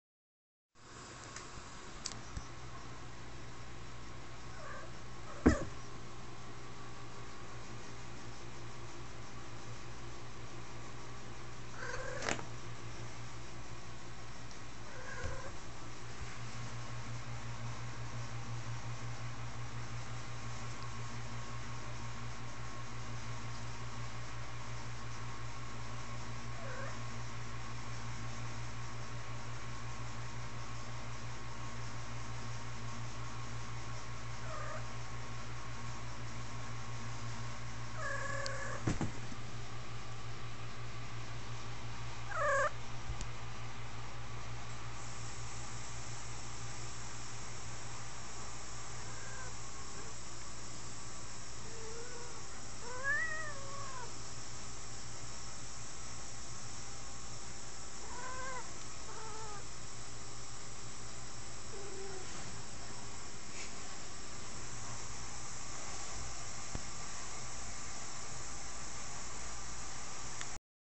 Mewp
The girls are feeling chatty.  Ignore the dish washer in the background o.o